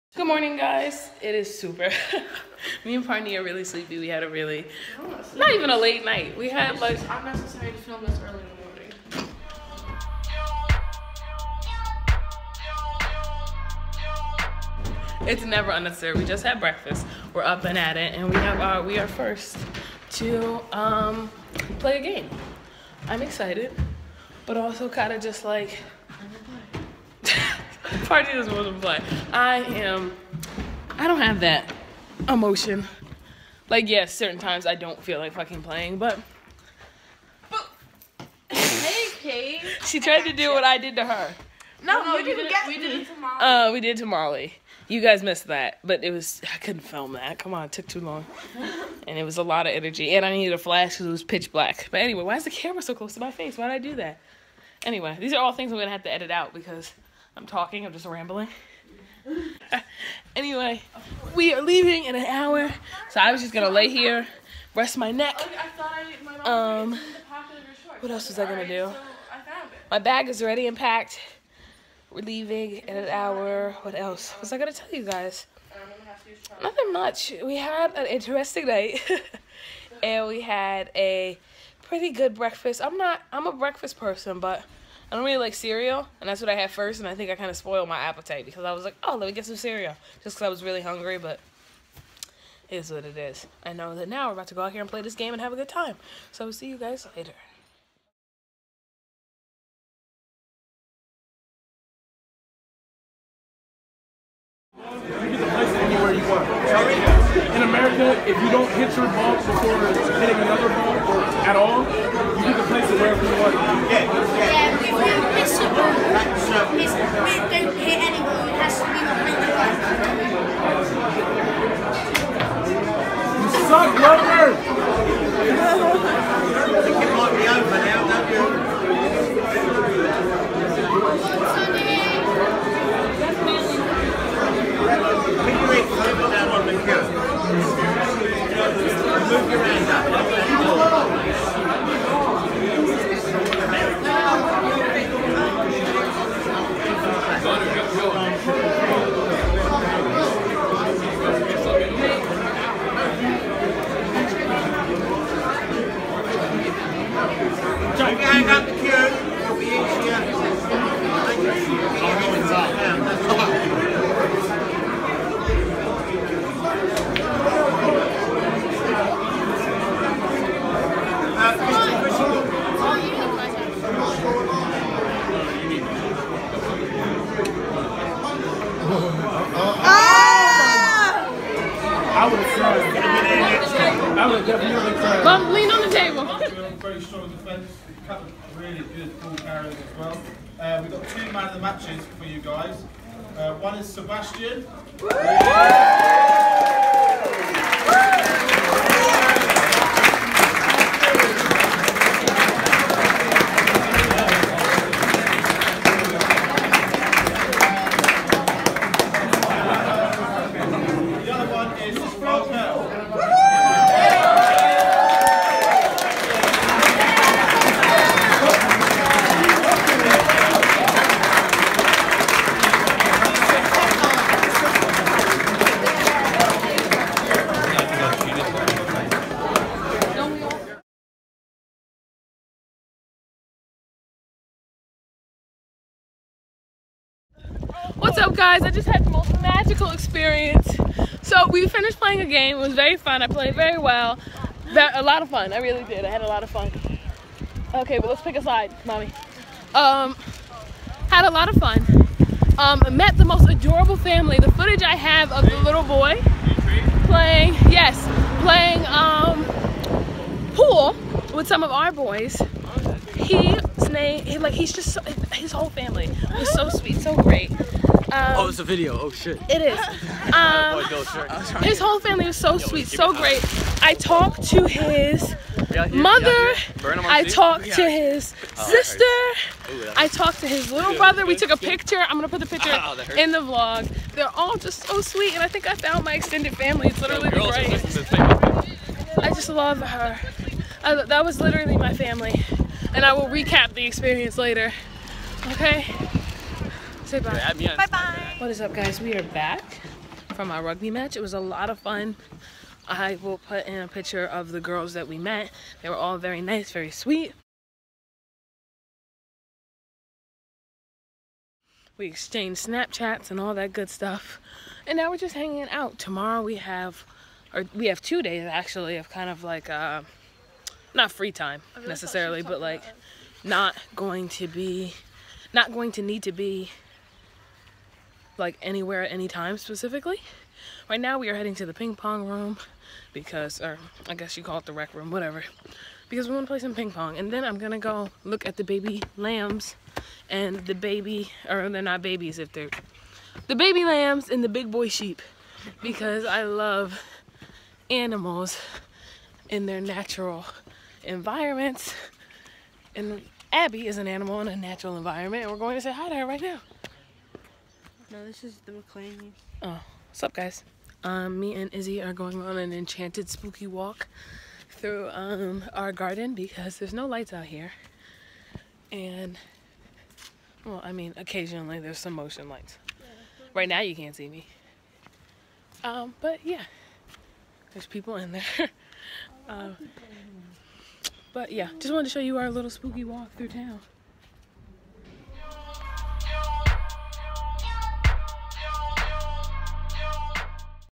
Long Lost Family 🇬🇧💙||Day 3 Rugby Tour Vlogs
I met so many cool people in England, they felt like family.